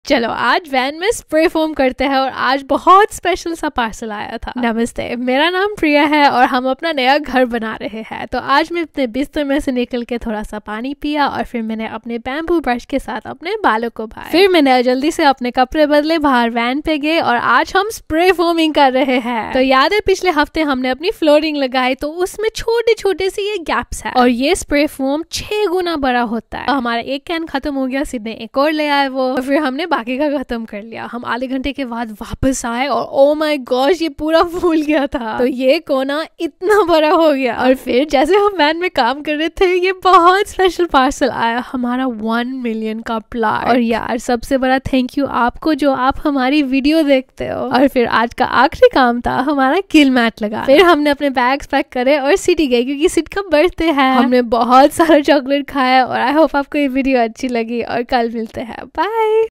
Spray foaming the van, Sid’s birthday & our 1 million plaque
This weekend it was Sid's birthday so we had a little getaway! Regarding the van, we did just a few things! So remember how we added the flooring last week, so today we had to add the spray foam insulation to the edges of the plywood boards, this is so that little insects don't make their way into the van. Then we added out kilmat, which is honestly amazing, I love it, it just changes the sound in the van. love you xx